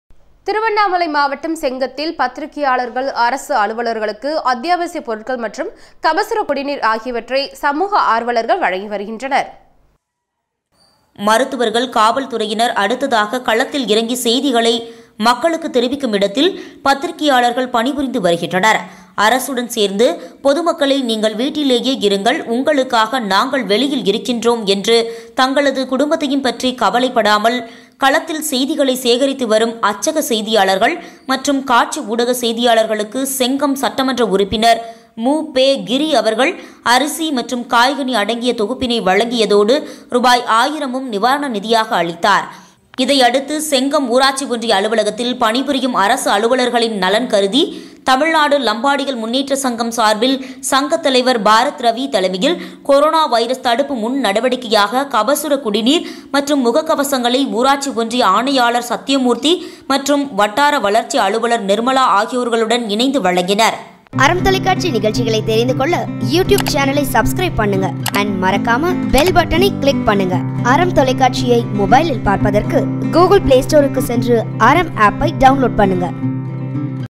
பத்திரிக்கையாளர்கள் அரசு அலுவலர்களுக்கு அத்தியாவசிய பொருட்கள் கபசுர குடிநீர் வழங்கி உதவி | ARAM NEWS
திருவண்ணாமலை மாவட்டம் செங்கத்தில் பத்திரிக்கையாளர்கள், அரசு அலுவலர்களுக்கு அத்தியாவசிய பொருட்கள் மற்றும் கபசுர குடிநீர் ஆகியவற்றை சமூக ஆர்வளர்கள் வழங்கி வருகின்றனர்.